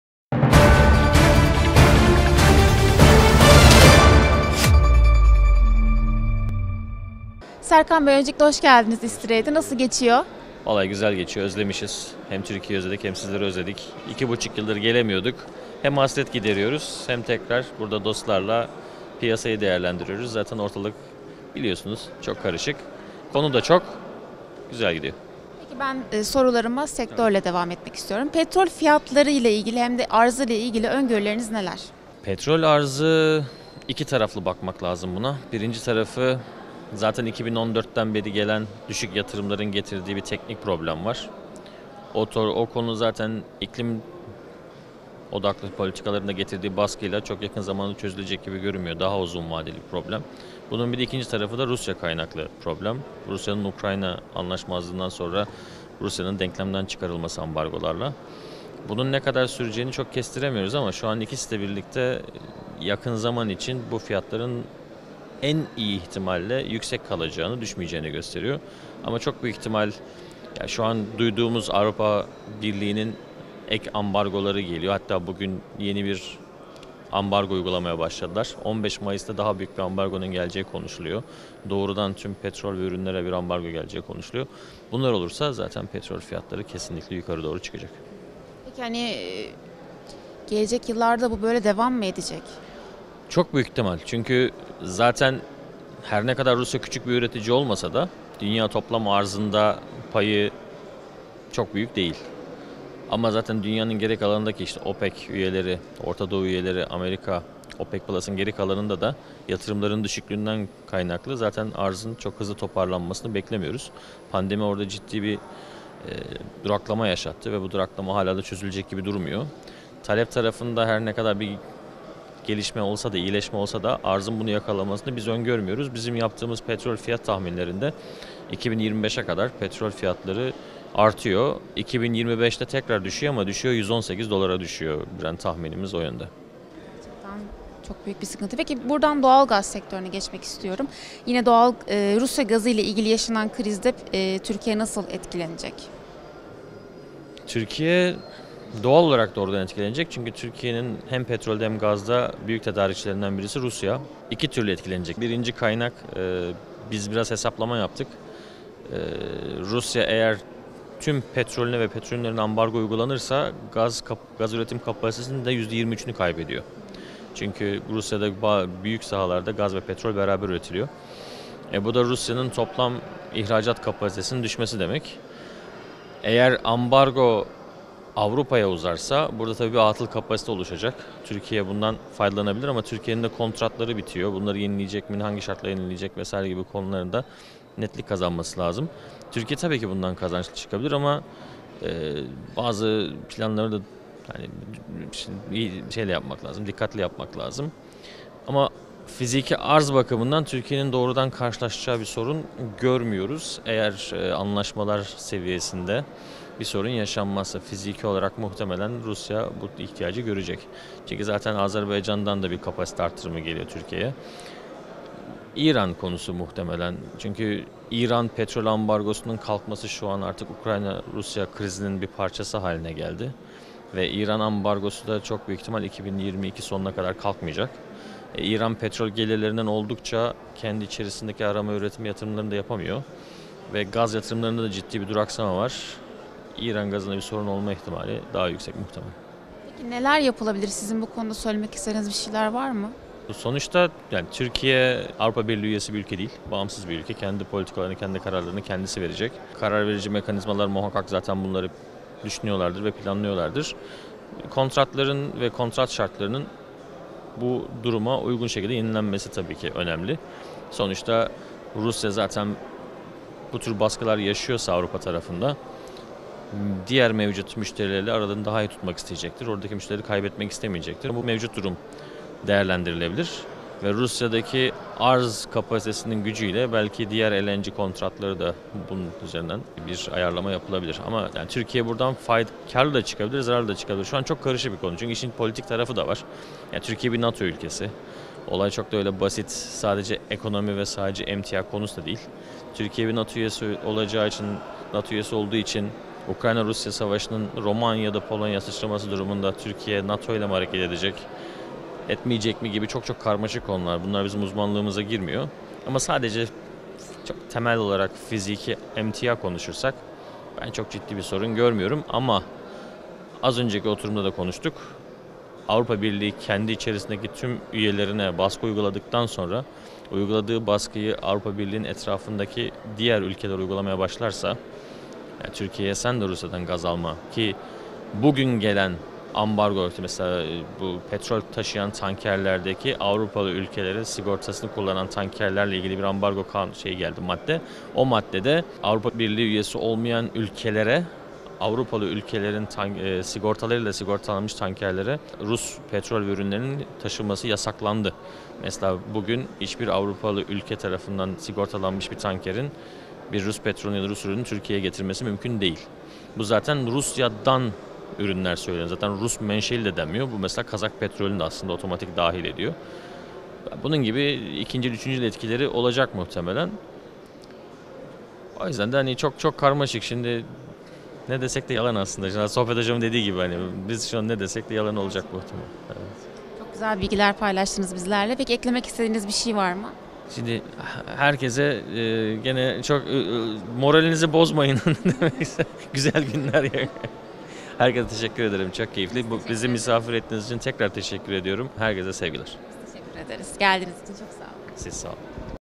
Serkan Şahin, "Petrol fiyatları artmaya devam edecek"
Energy Aspects Upstream Petrol & Doğalgaz Bölümü Kıdemli Uzmanı Serkan Şahin, ISTRADE 2022'de Petroturk TV Muhabiri Özge Esen'in sorularını yanıtladı.

Petroturk TV
Enerjinin Haber Merkezi